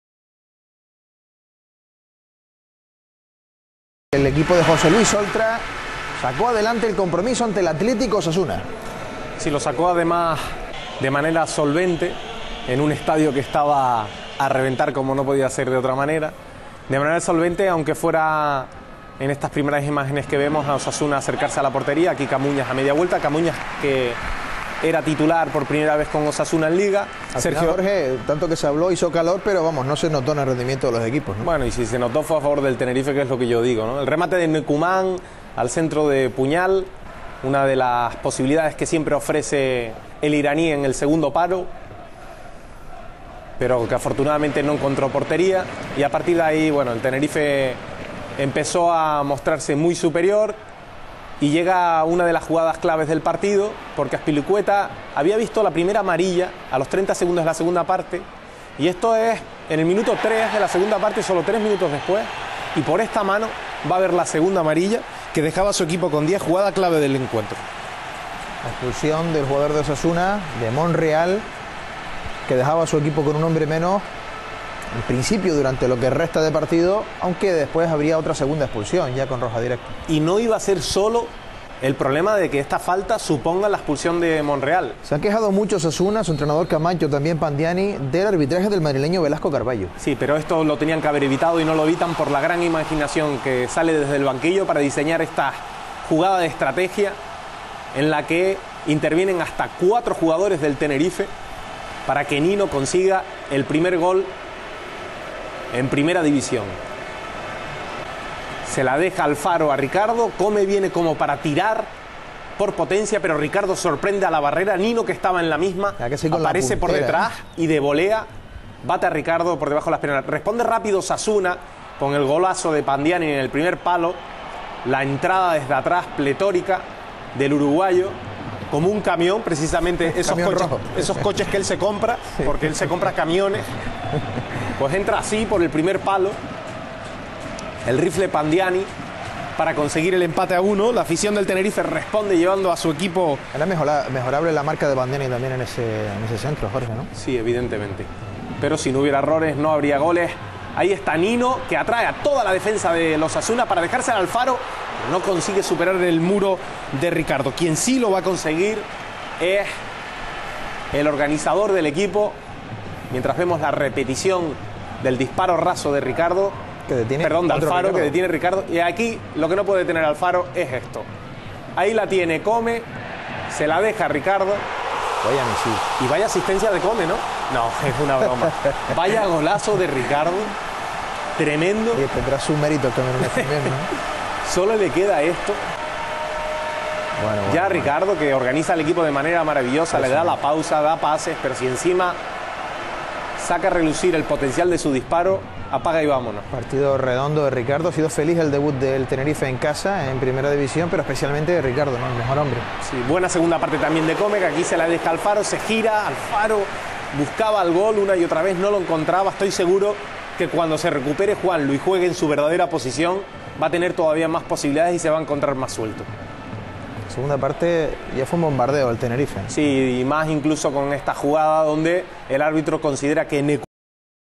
CD Tenerife 2-1 CA Osasuna
InstintoBlanquiazul.Es te ofrece el resumen del CD Tenerife-CA Osasuna correspondiente a la 2ª jornada de la Liga BBVA 09/10. Imágenes de RTVC